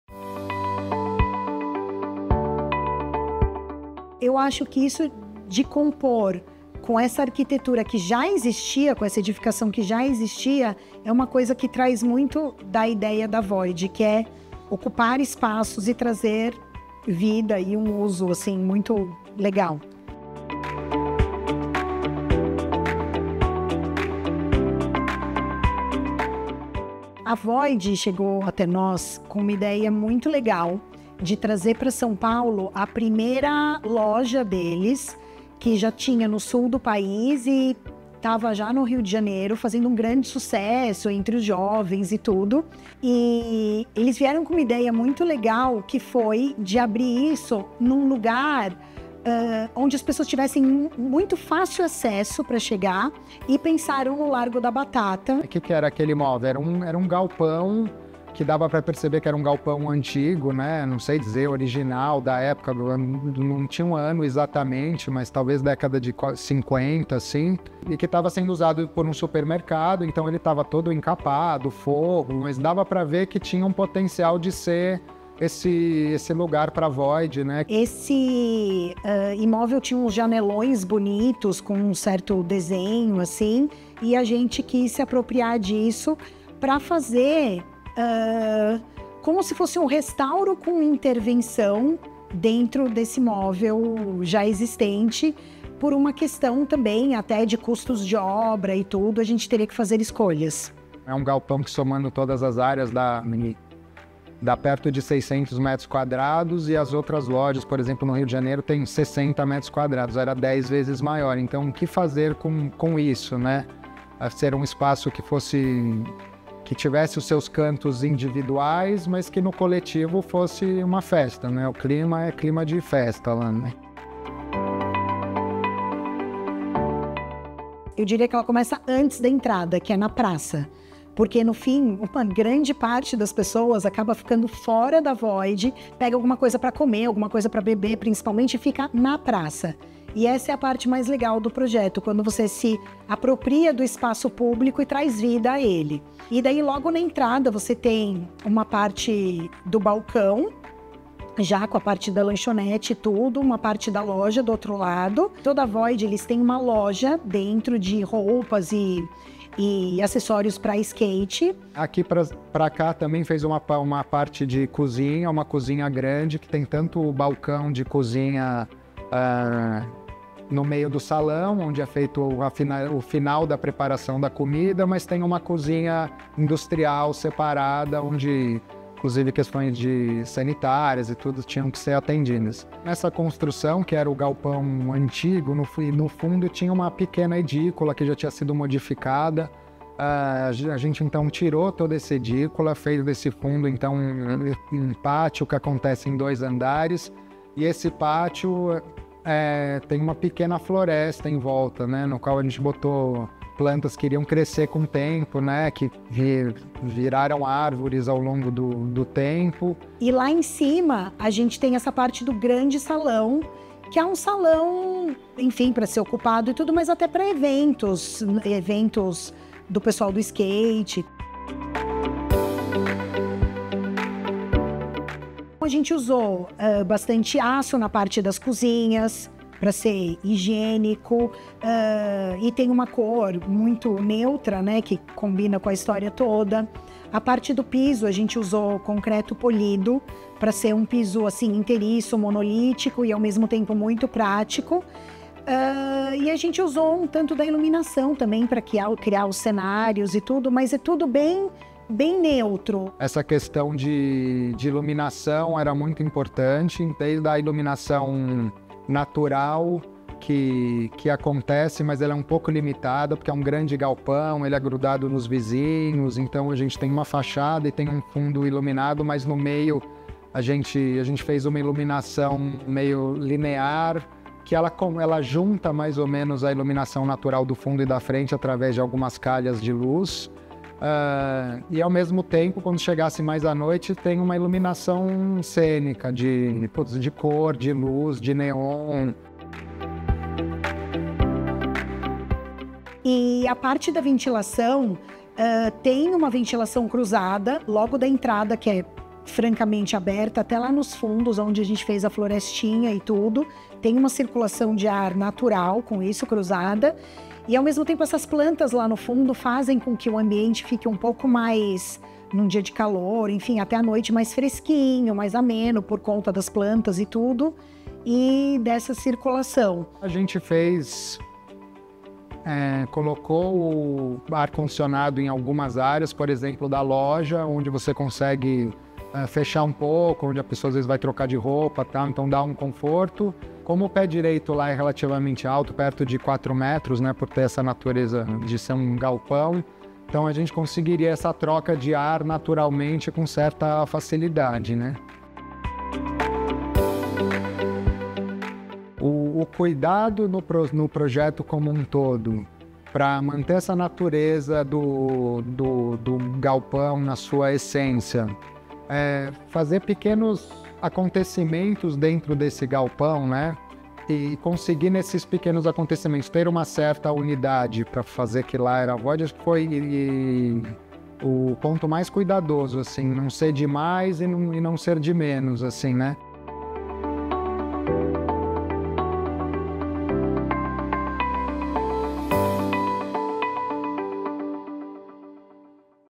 Void Largo da Batata - Arkitito Arquitetura | Galeria da Arquitetura
No cenário cosmopolita de São Paulo, o escritório de arquitetura Arkitito projetou um estabelecimento que caiu no gosto dos jovens paulistanos: a Void. Estabelecida como um espaço de criatividade e energia, a casa noturna contribuiu para a revitalização do Largo da Batata — um ícone da cidade que ganhou uma nova vida depois da construção.

Em entrevista à Galeria da Arquitetura, os irmãos e arquitetos Chantal e Tito Ficarelli contam que a jornada começou com a proposta de trazer para São Paulo a atmosfera vibrante que já conquistara o sul do país e o Rio de Janeiro. O desafio? Transformar um antigo galpão, outrora abrigo de um supermercado, no vibrante epicentro da cultura jovem.